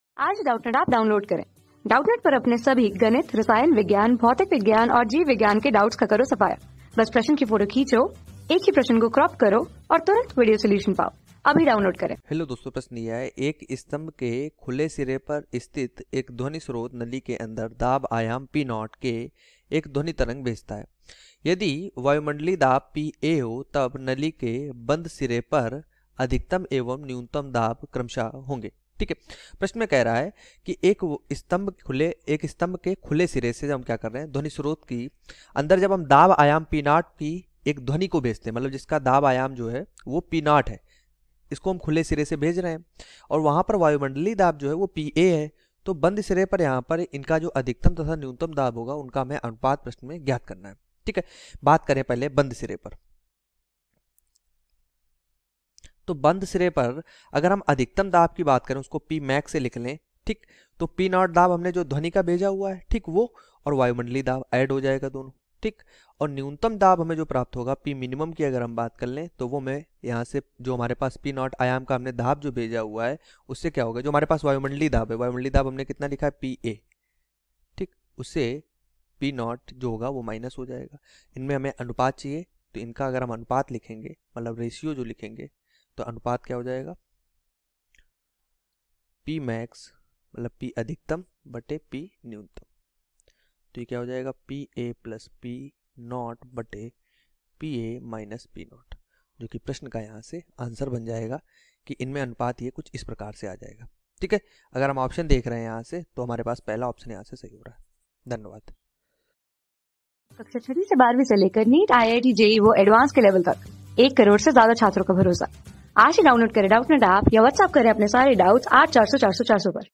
एक स्तम्भ के खुले सिरे पर स्थित एक ध्वनि स्रोत नली के अंदर दाब आयाम P_(0) की एक ध्वनि तरंग भेजता...
एक स्तम्भ के खुले सिरे पर स्थित एक ध्वनि स्रोत नली के अंदर दाब आयाम P_(0) की एक ध्वनि तरंग भेजता है। यदि वायुमंडलीय दाब P_(A) है तब नली के बन्द सिरे पर अधिकतम व न्यूनतम दाब क्रमशः होंगे
Class: 12
Subject: PHYSICS
Chapter: ध्वनि एवं तरंग
Board:NEET

👉 Have Any Query? Ask Us.